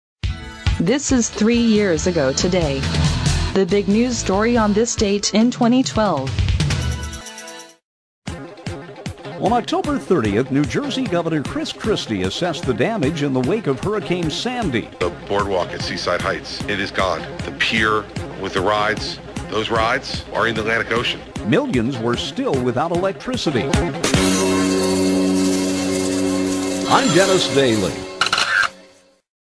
THREE YEARS AGO TODAY -- OCTOBER 30
DENNIS DAILY goes back into the archives for the top news story on this date in 2012.